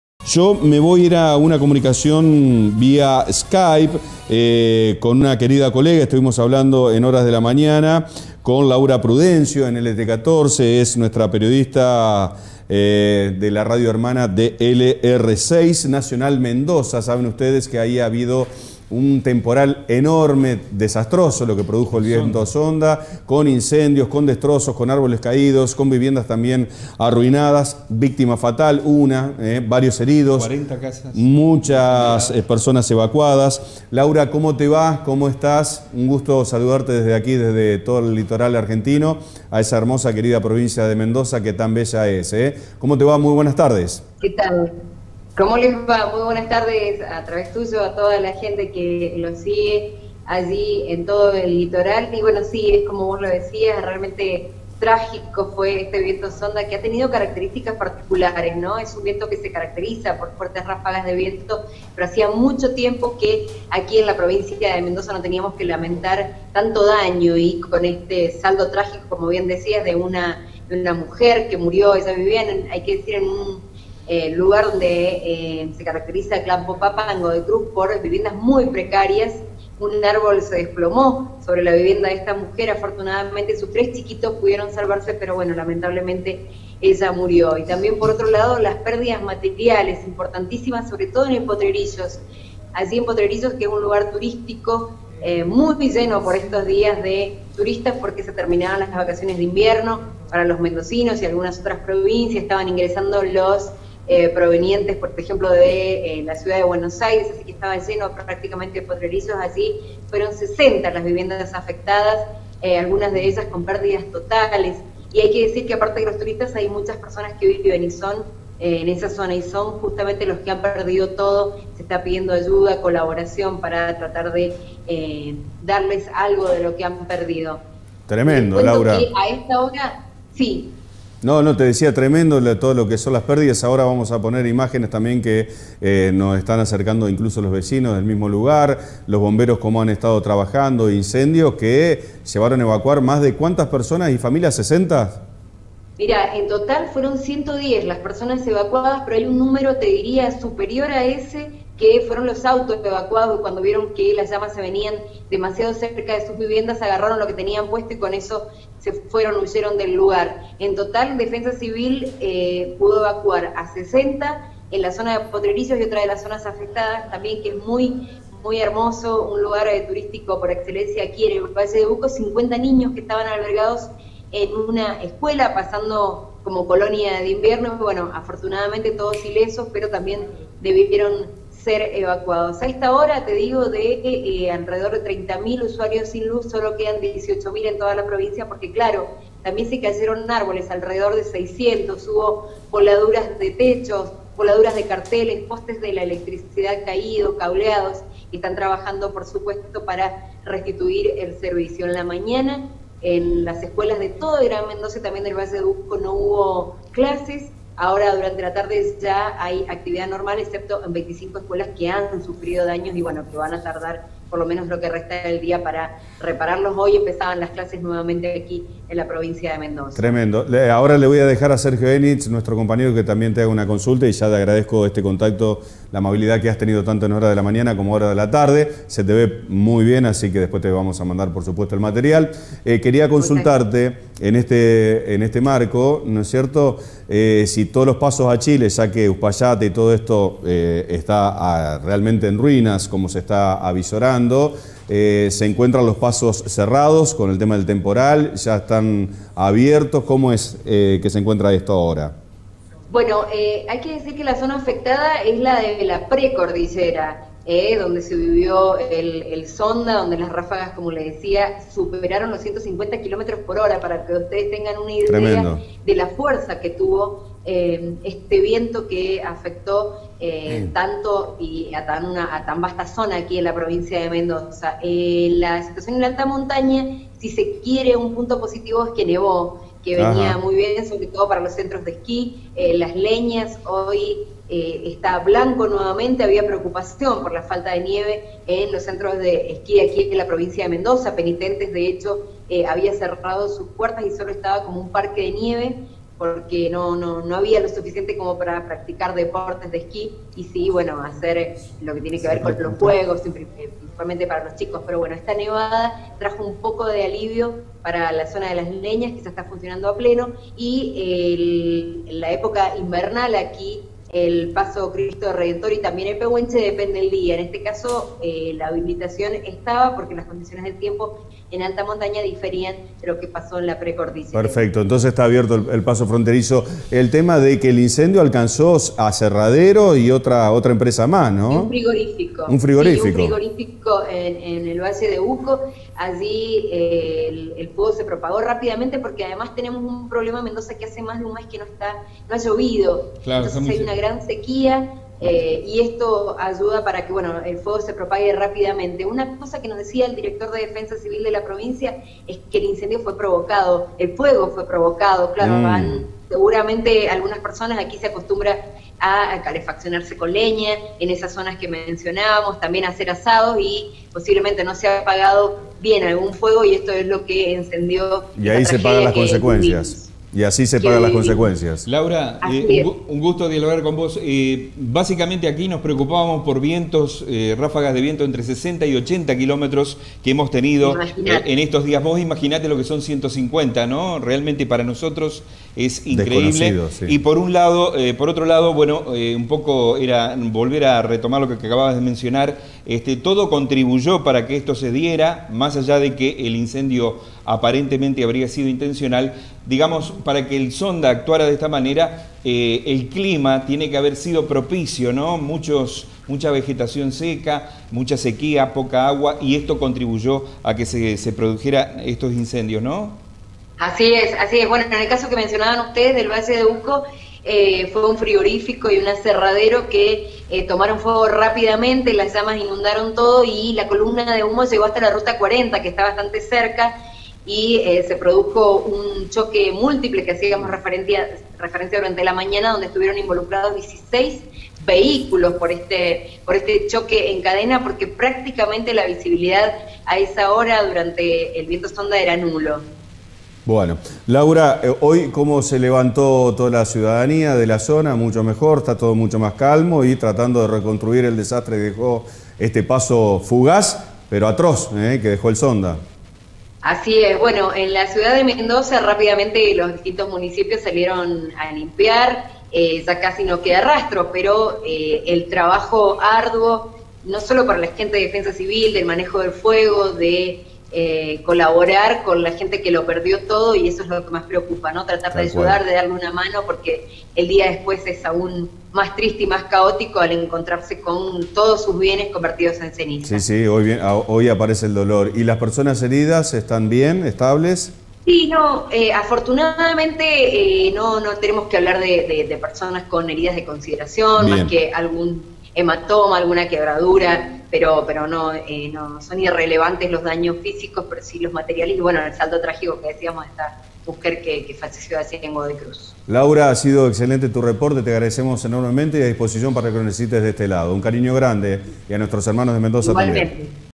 El zonda pasó con fuerza
Hablamos con nuestra colega, Laura Prudencio, periodista de LR6 Radio Nacional Mendoza, sobre rvacuados, cabañas quemadas y árboles caídos en el norte de Mendoza.
Durante la jornada del sábado, la zona de Potrerillos fue afectada por viento zonda, con un alto nivel de destrucción. Se quemaron unas 30 cabañas.
Personal del Plan de Manejo del Fuego debió actuar en forma conjunta con Defensa Civil  y se informó que se quemaron en un 100% al menos 30 cabañas.